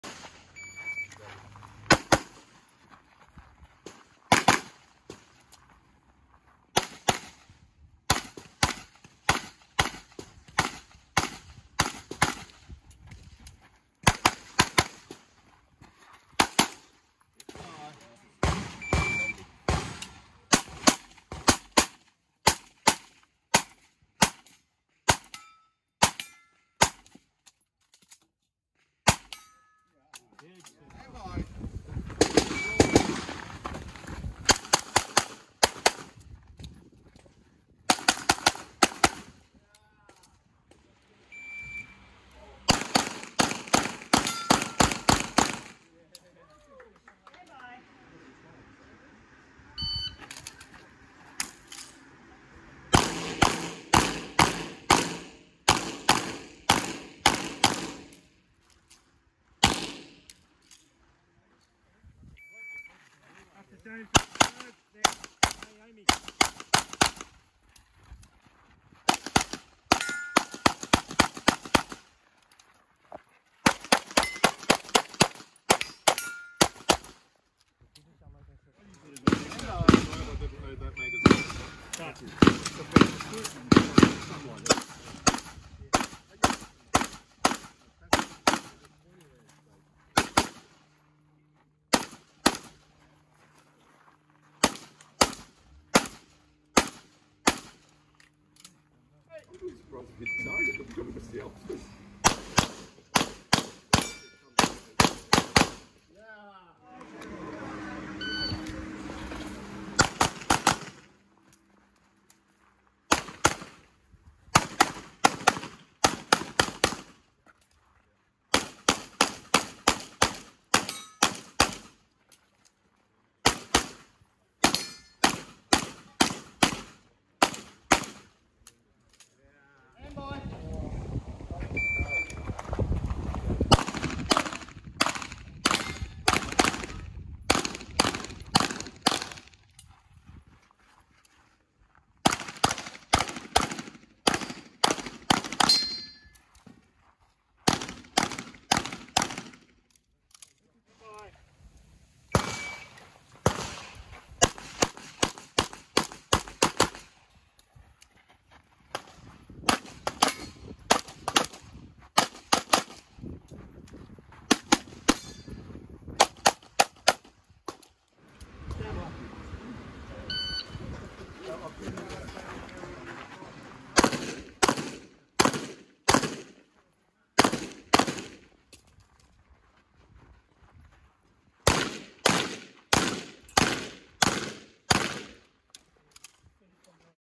2021 NT IPSC State Titles - James Phegan
Grandmaster and junior champion at Northern Territory IPSC handgun state championship